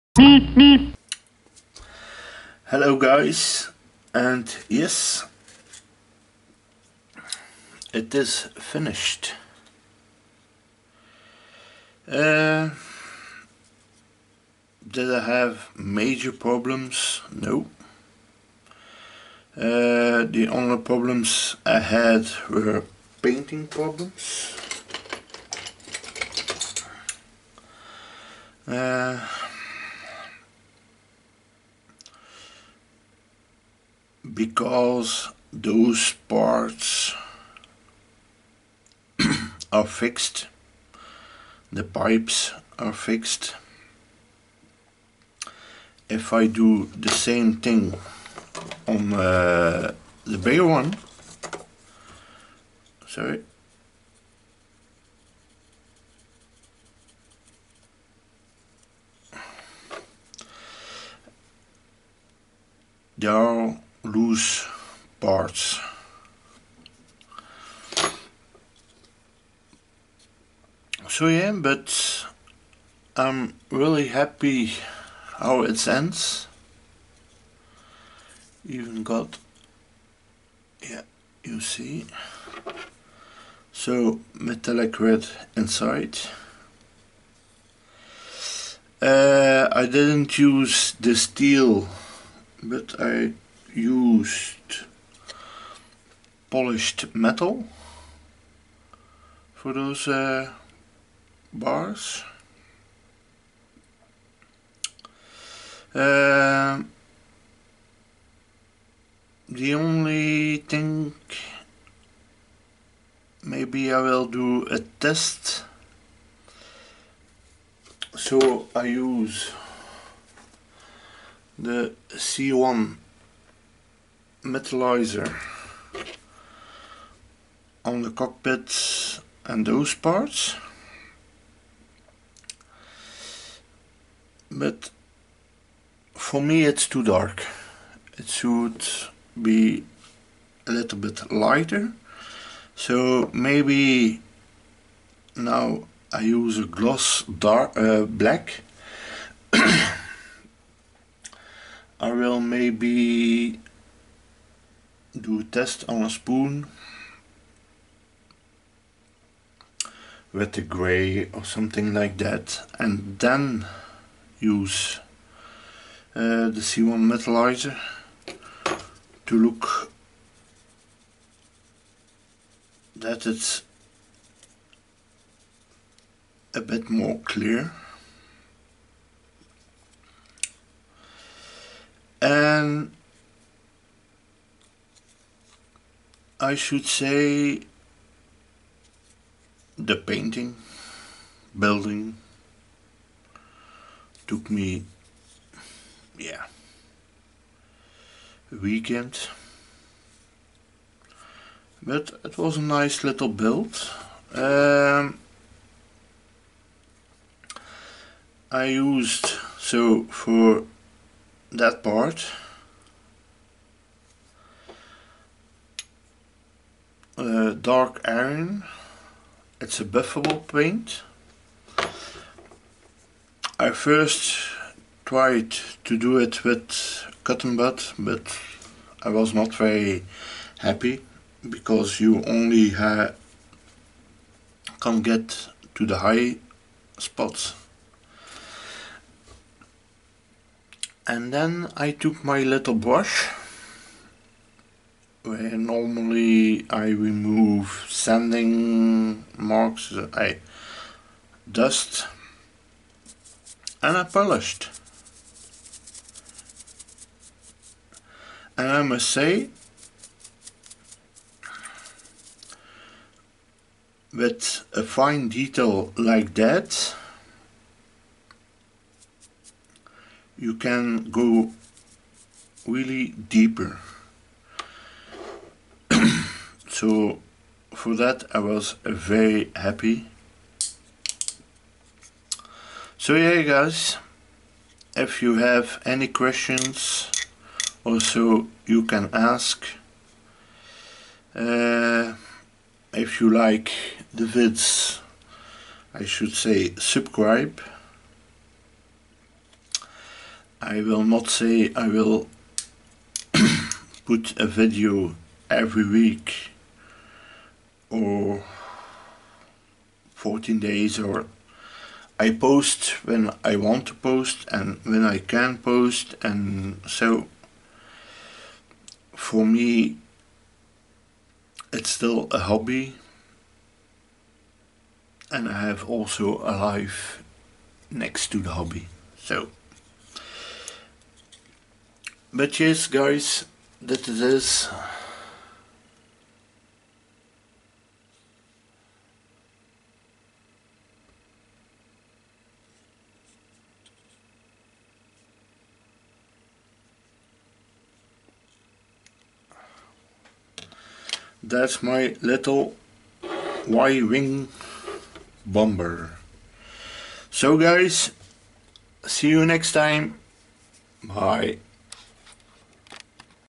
Metal Y-wing
Here you see how it went with the metal paint job on the little y-wing